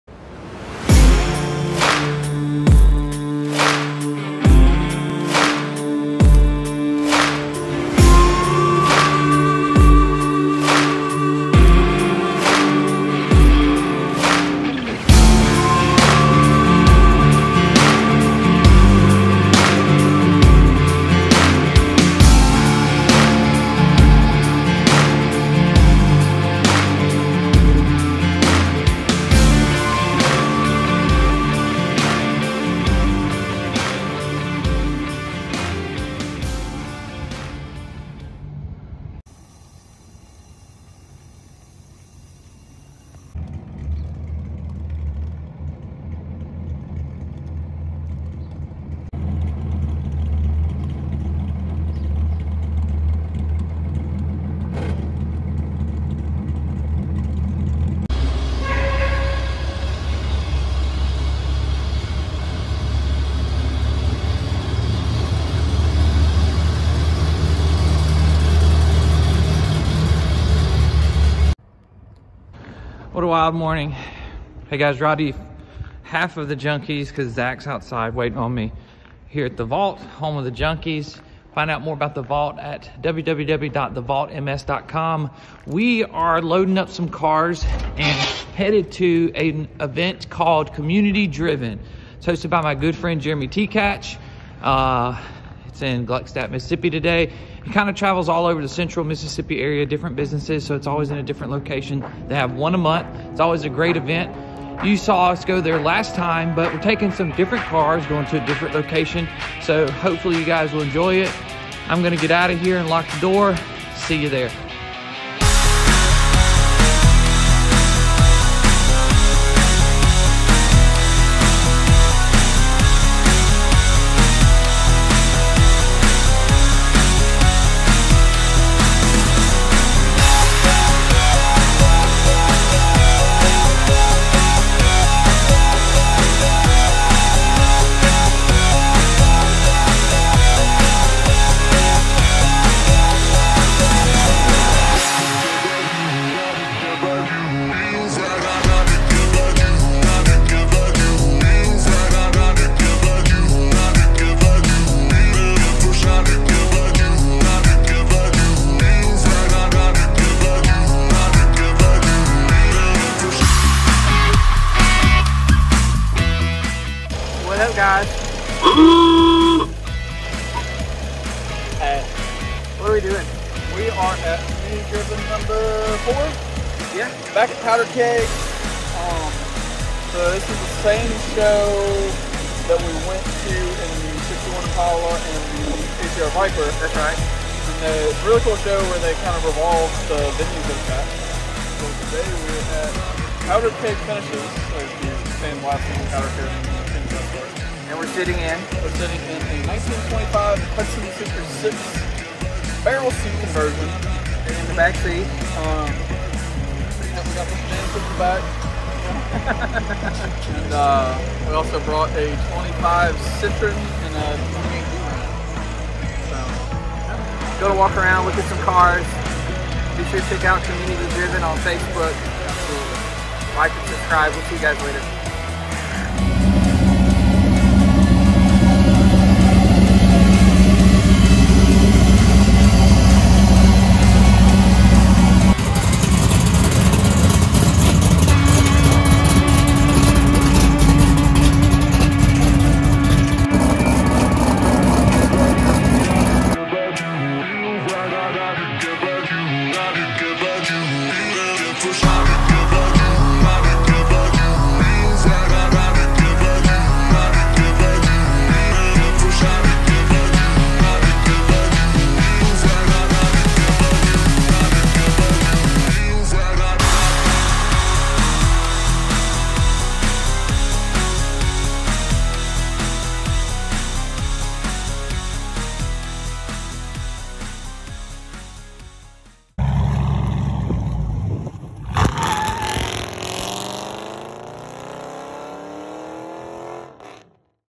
Junkies Re Do Community Driven
It's back to Community Driven, but this time at different location, with different cars, and a little something something at the end. You have to watch the whole thing to see it all!!!